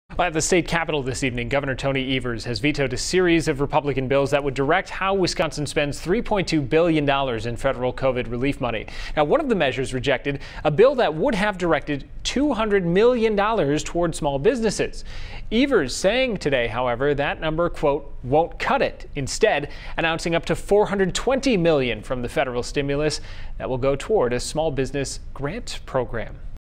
Gov. Evers vetoes Republican COVID-19 relief package
Gov. Evers is officially vetoing a package of 11 bills championed by Republicans that passed through the Wisconsin legislature last week.